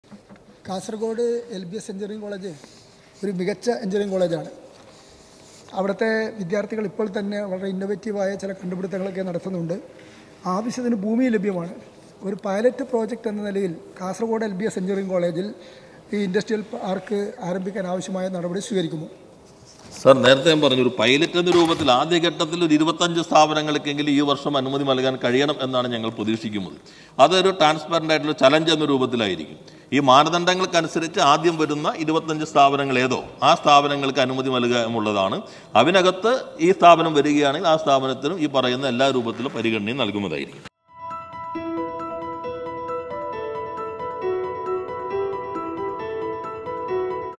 മാനദണ്ഡങ്ങൾക്കനുസരിച്ച് ആദ്യം വരുന്ന 25 സ്ഥാപനങ്ങളിൽ പൈലറ്റ് രൂപത്തിൽ ഇന്‍ഡസ്ട്രിയല്‍ പാർക്ക്
കാസർഗോഡ് LBS എഞ്ചിനിയറിംഗ് കോളേജിൽ  ഇൻഡസ്ട്രിയൽ പാർക്ക് | കോളേജ് ക്യാമ്പസുകളില്‍ ഇന്‍ഡസ്ട്രിയല്‍ പാര്‍ക്കുകള്‍ | LBS College of Engineering
 | Campus Industrial Park

ചോദ്യോത്തരവേള | Question Hour | KLA 15 | Session-11 | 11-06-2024 |  Industrial Park in college campus of Kerala

P. Rajeev | പി. രാജീവ് | Minister  for law,  industries and coir | നിയമ, വ്യവസായ, കയർ വികസന വകുപ്പ് മന്ത്രി

•  C. H. Kunhambu | സി.എച്ച്.കുഞ്ഞമ്പു | ഉദുമ | Udma Assembly constituency